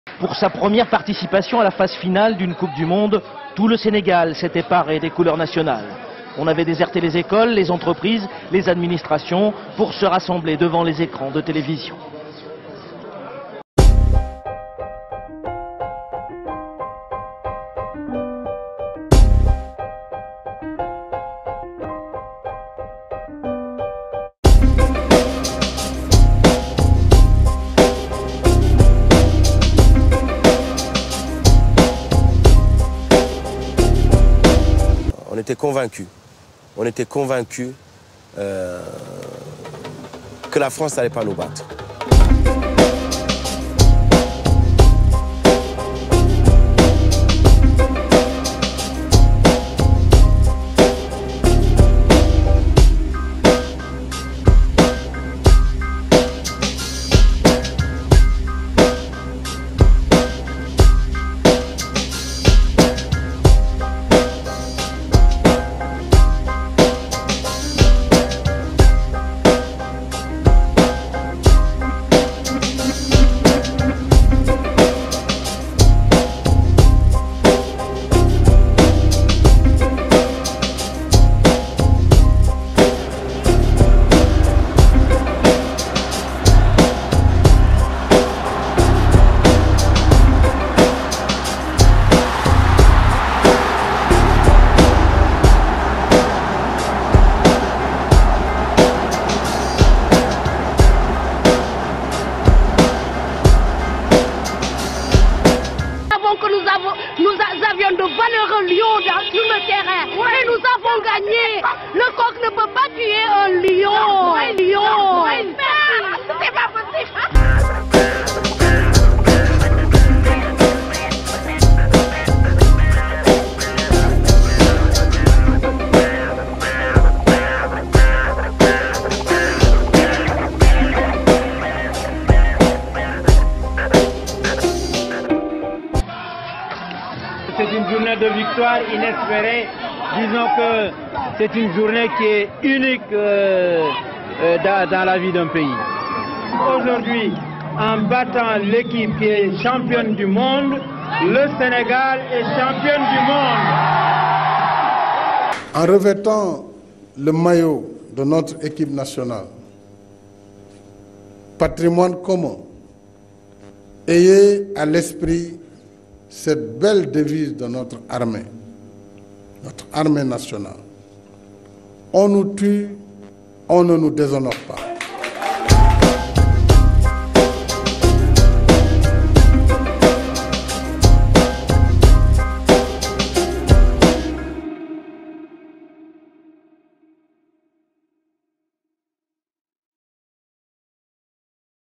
Il était une fois l'épopée des lions à la Coupe du Monde...
Mine de Rien 16 ans déjà depuis la Victoire Historique du Sénégal face à la France durant la Coupe du Monde 2002, Revivez le Match Commenté par @Sétanal!